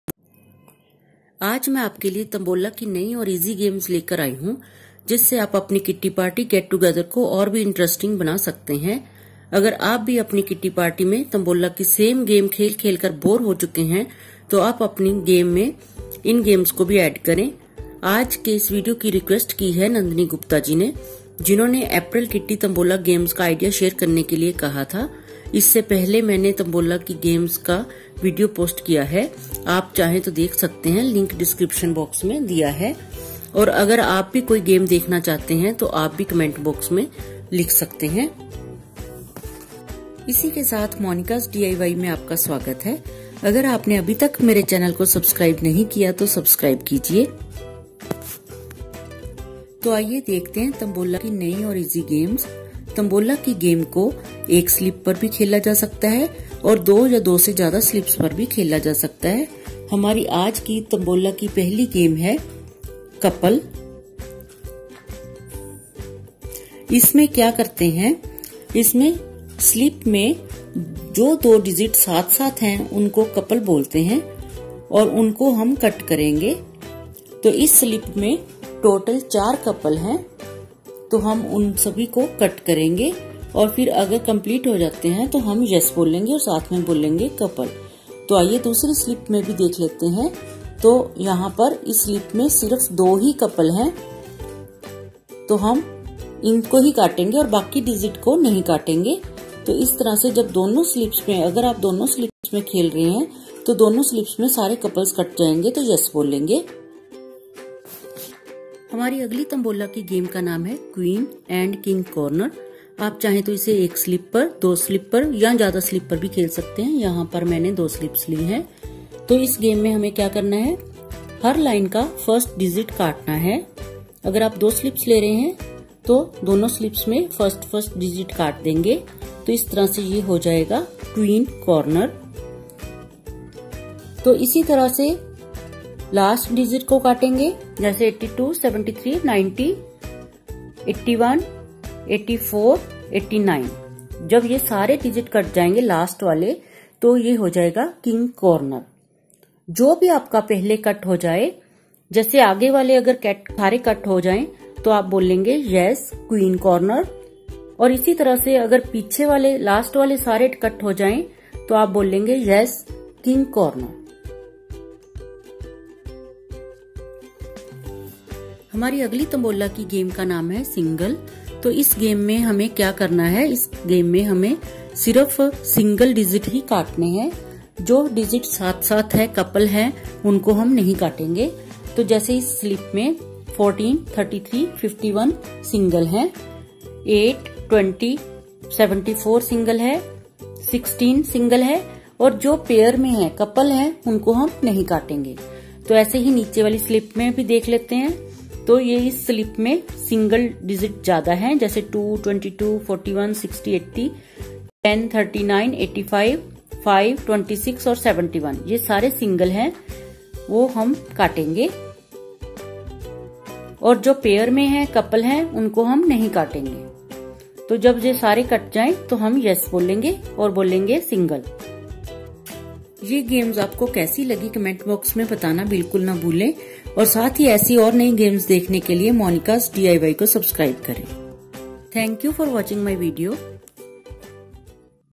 3 New Tambola Games for Kitty Party - Monikazz DIY
Sharing with you 3 New Tambola Games for Kitty Party with full details. New and different tambola games ideas for Kitty party is fun to play. Kitty games is liked by all ladies in Kitty Party as kitty tambola.

Watch in hindi 3 New Tambola Games for Kitty Party - monikazz DIY.

Do give your valuable feedback about the video, any suggestion and related videos you would like to see in my future uploads.